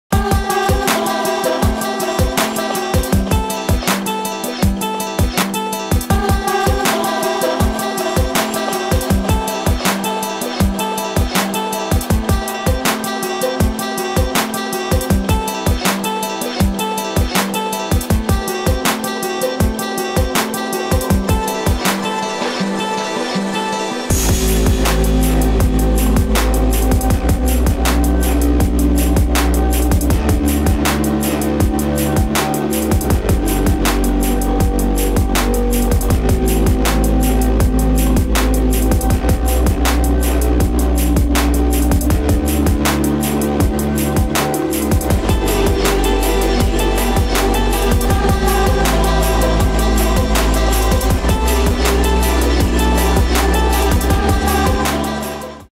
Shredding the track
Getting faster at this track I set in North Van